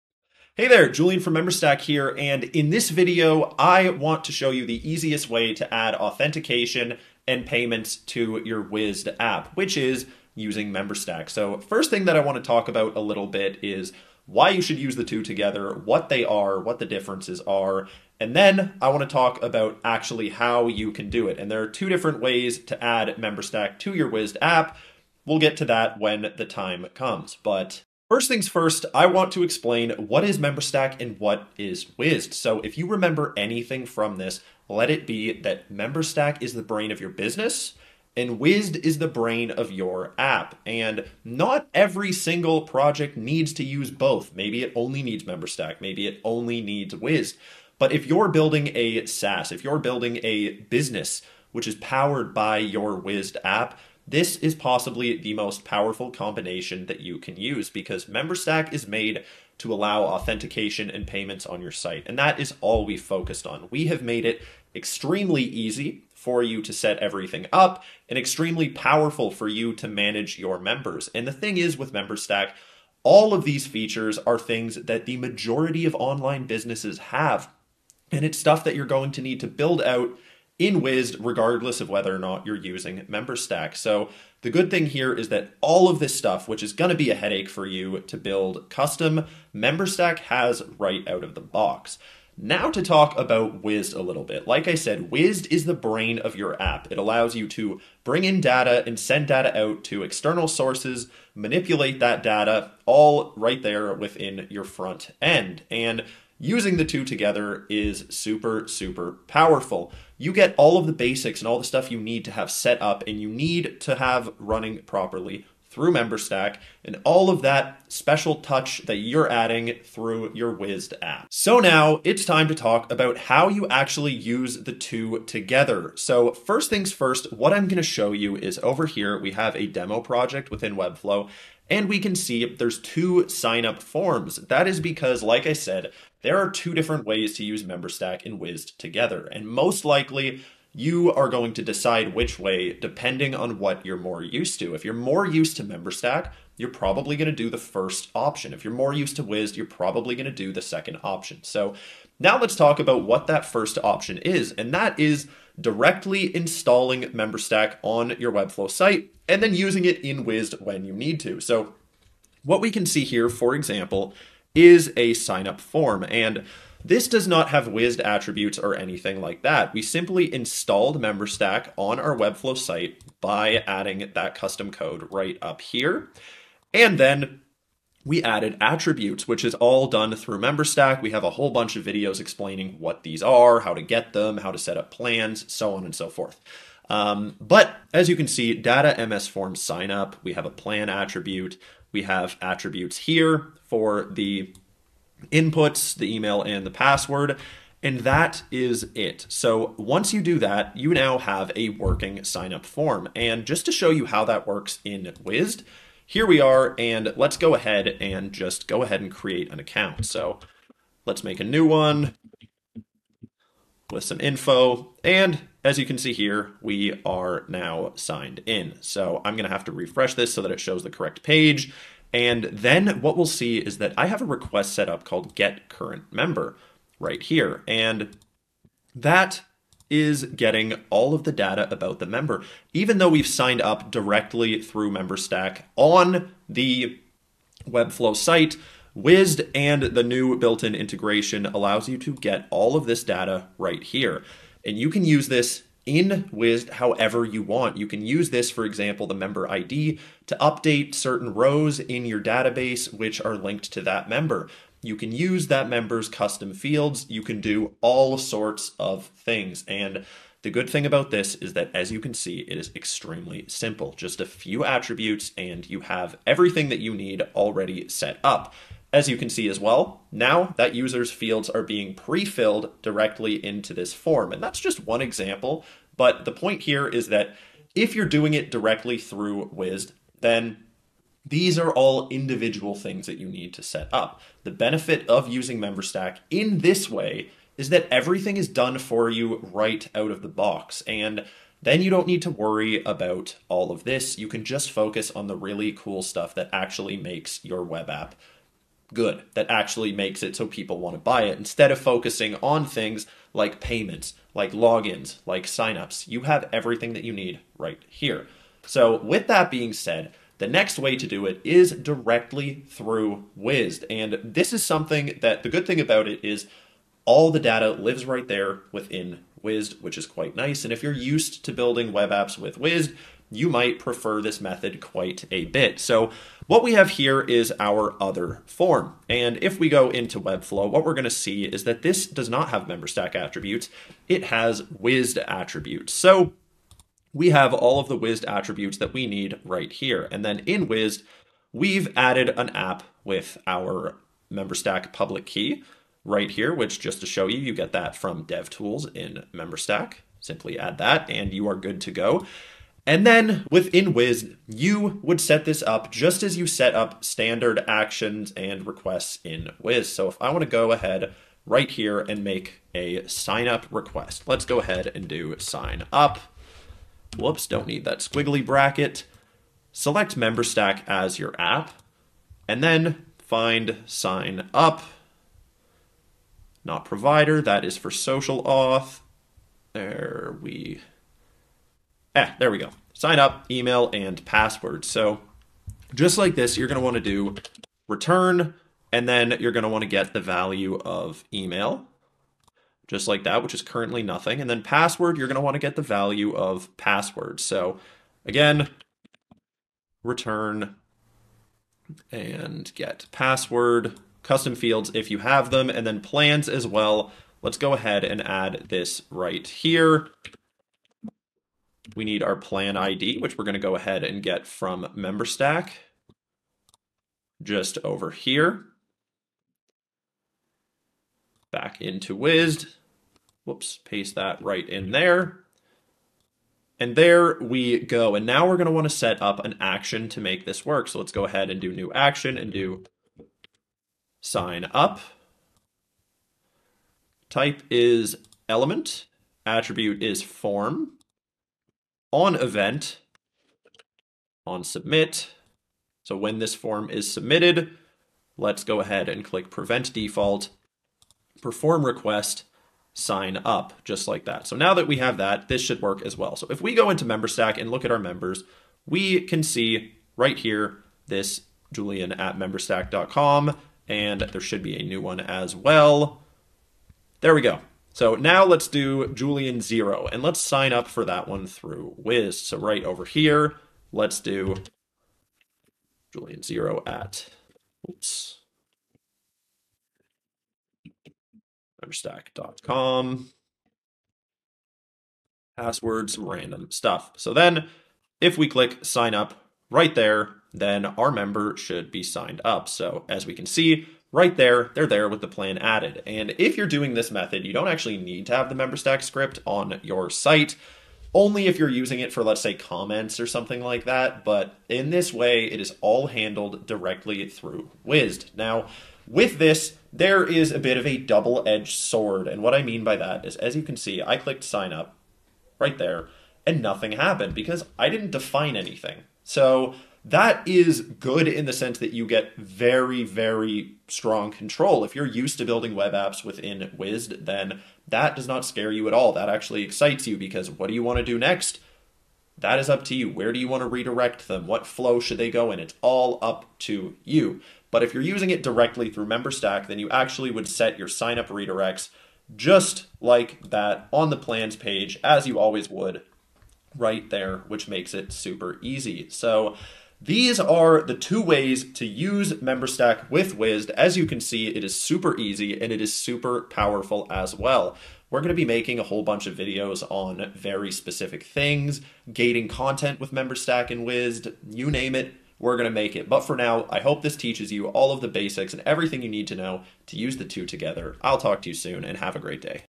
Memberstack & Wized | The Easiest Way To Set Up Auth & Payments
Wized is a very powerful tool - but, there are some basic things that every monetised web app needs which can be quite a pain to properly set up.

Introducing the native Memberstack & Wized integration - the easiest way to turn your web app into a profitable business.

In this video, we will show you how to set everything up - both directly through Memberstack, and within Wized.